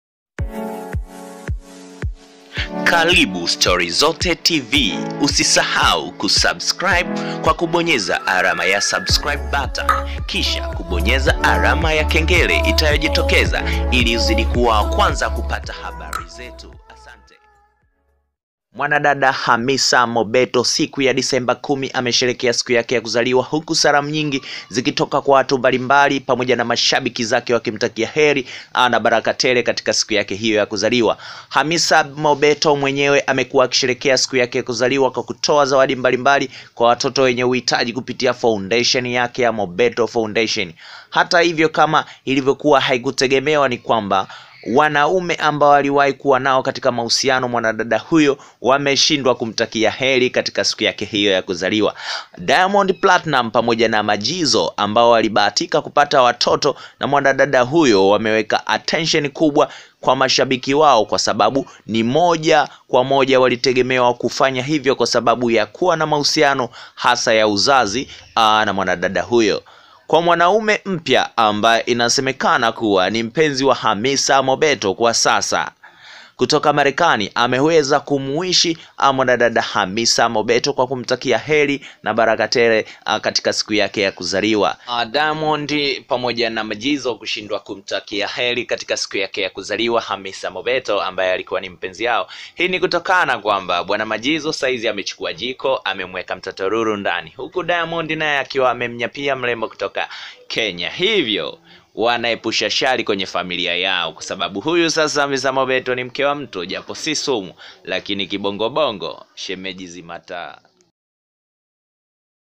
Diamond Platnumz na Majizo waungana kumsusa Hamisa Mobeto
Diamond na Mkurugenzi mtendaji wa EFM Majizo wamemkalia kimua Hamisa Mobeto mara baada ya kutomtakia kheri ya siku yake ya kuzaliwa.